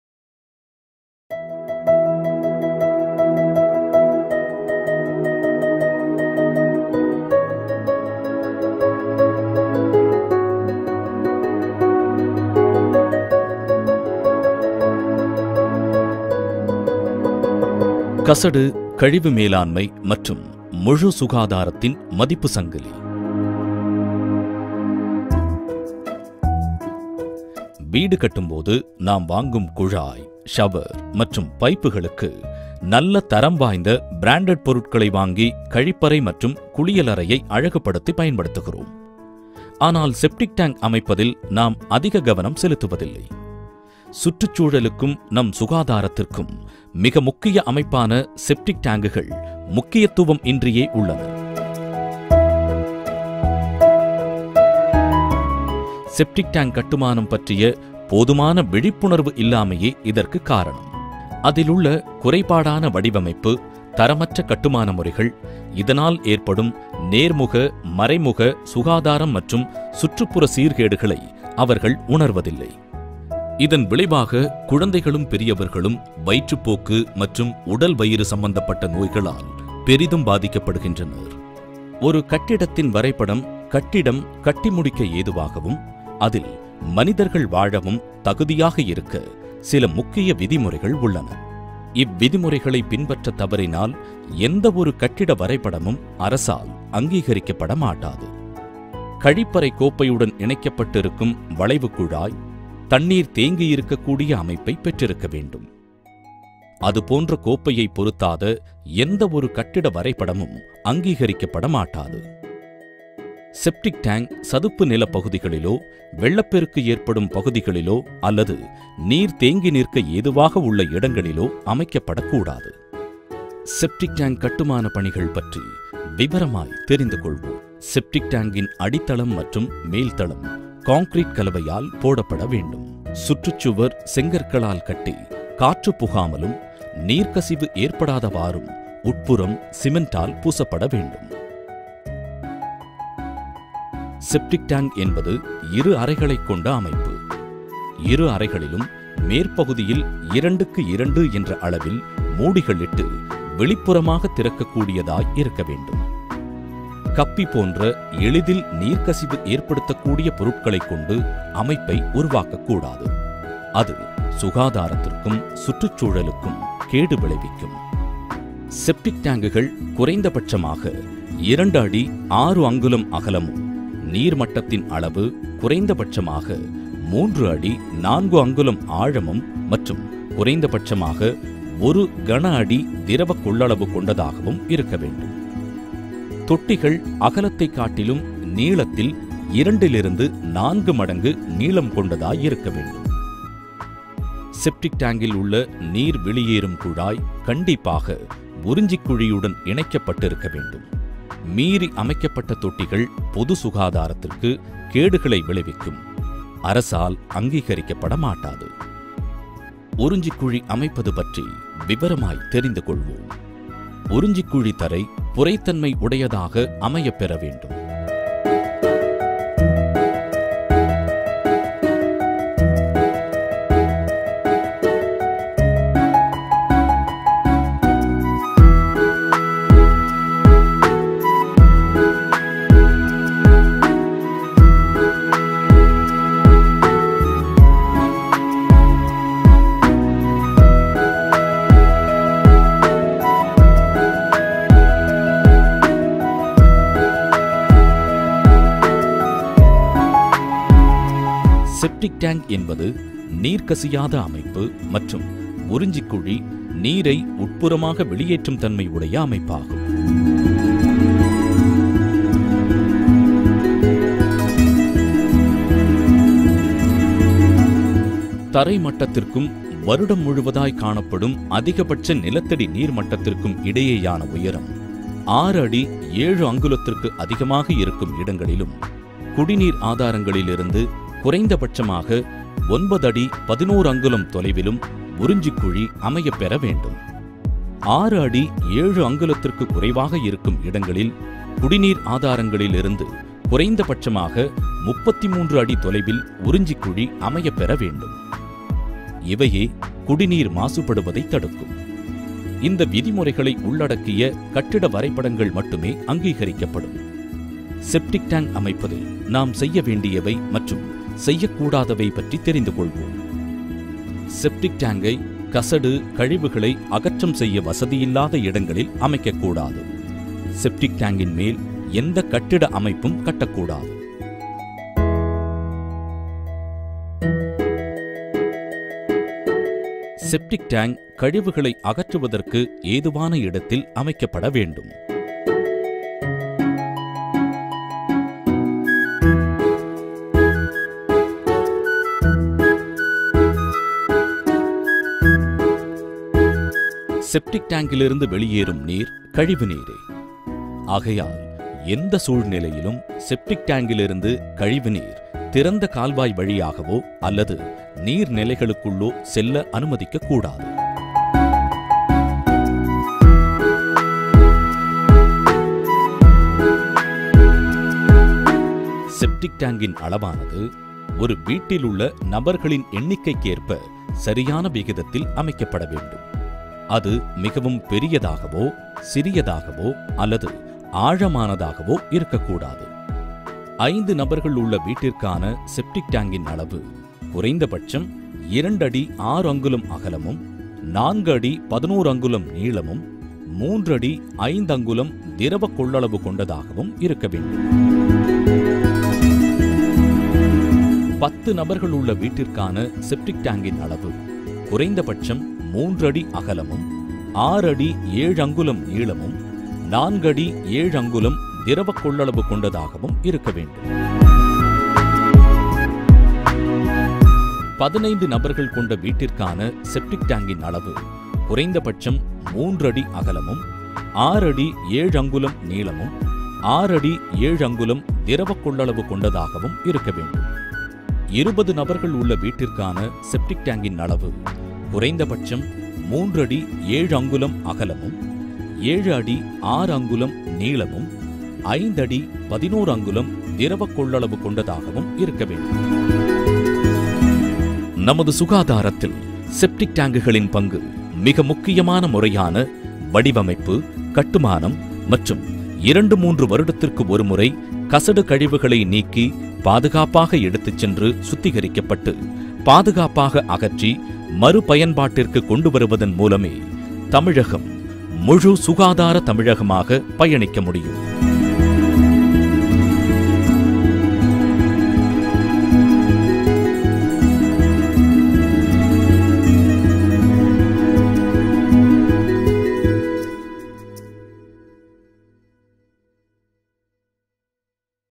Construction of Septic tank and soak pit as per Tamilnadu Government procedure Tamil
Tamil Nadu government released this animation video to spread awareness about the construction of septic tank and soak pit.